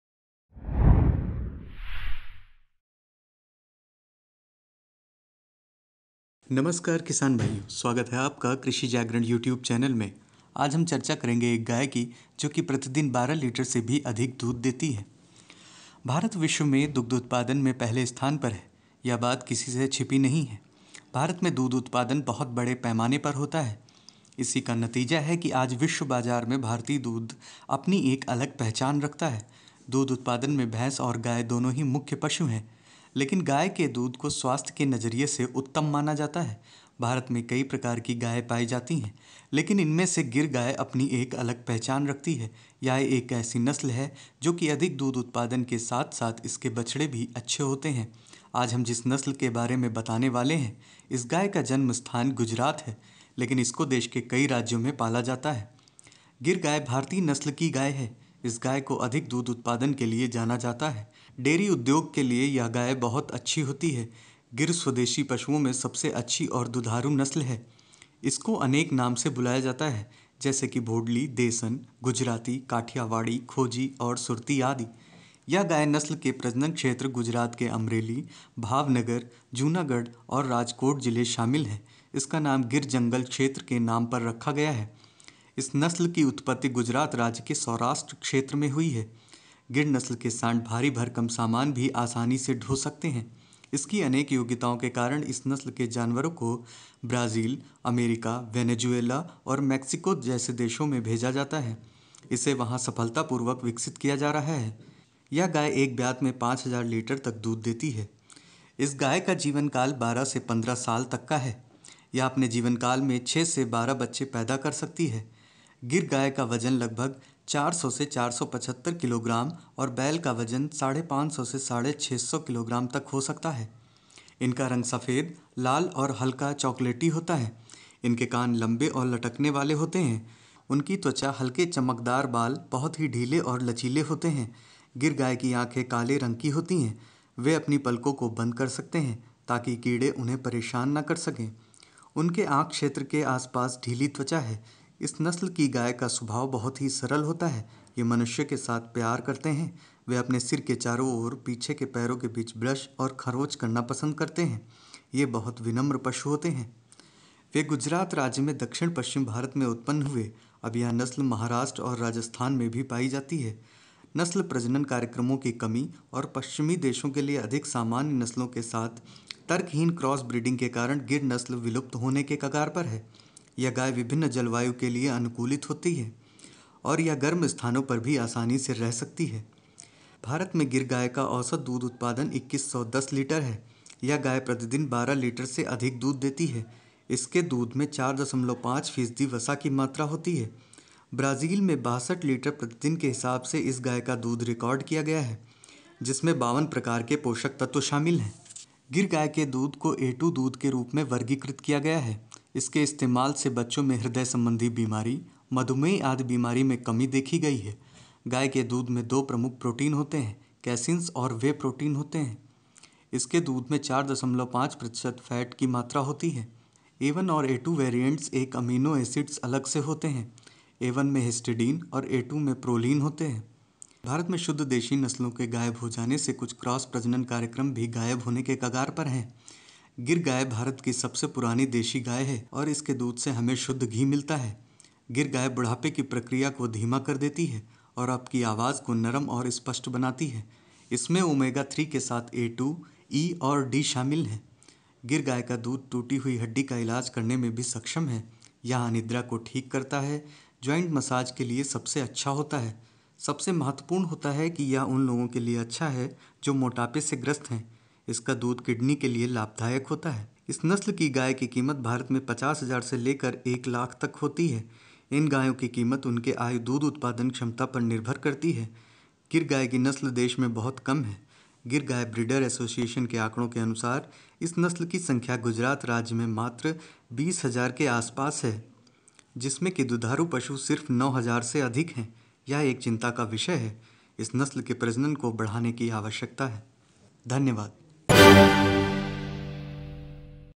यह गाय प्रतिदिन देती है 12 लीटर से अधिक दूध//This Cow Gives 12 liters milk per day//Krishi Jagran
भारत विश्व में दुग्ध उत्पादन में पहले स्थान पर है. यह बात किसी से छिपी नहीं है कि भारत में दूध उत्पादन बहुत बड़े पैमाने पर होता है. इसी का नतीजा है कि आज विश्व बाजार में भारतीय दूध विश्व बाजार में अपनी एक अलग पहचान रखता है. दूध उत्पादन में भैंस और गाये दोनों ही मुख्य पशु हैं. लेकिन गाये को दूध को स्वास्थ्य के नजरिए से उत्तम माना जाता है. भारत में कई प्रकार की गाये पायी जाती है |